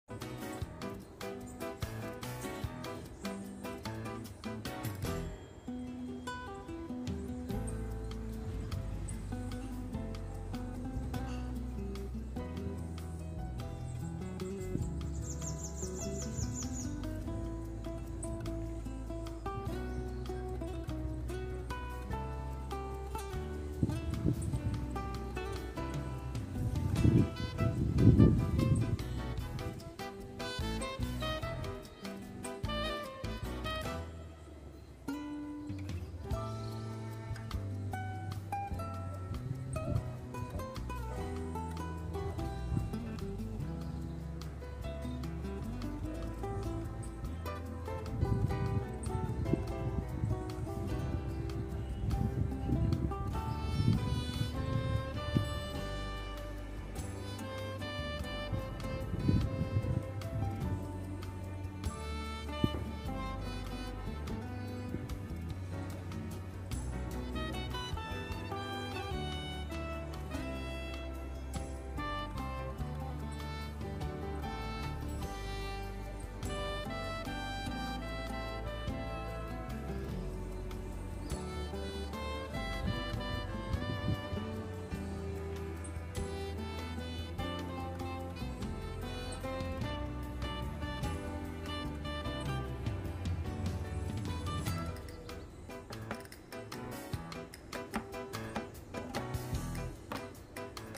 Working on the third quarter of the Tai Chi 24 step routine
Divide each difficulty into as many parts as feasible and necessary to resolve it. •Rene Descartes•

When you are faced with a task that seems impossible, break it down into smaller, more manageable steps. try to tackle at least one of these steps daily and let the momentum help you reach your goal 😊💚